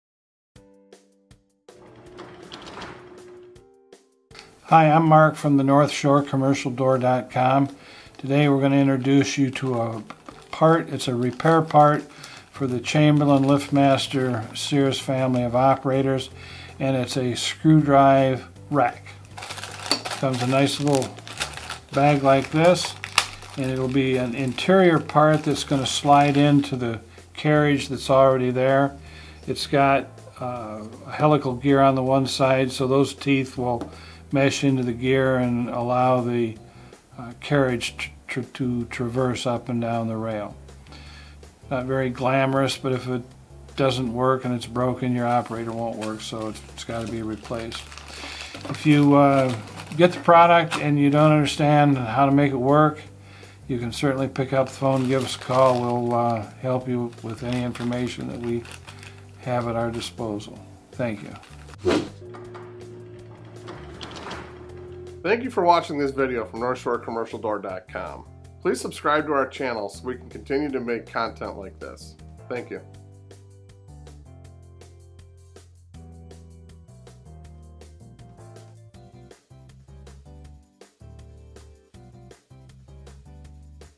Liftmaster Chamberlain 81C168 screw drive carriage rack
Works on Liftmaster, Sears Craftsman and Chamberlain garage door opener rails from 1995 to present.

Works in conjunction with carriage trolley part number 41C4677

NorthShoreCommercialDoor.com Production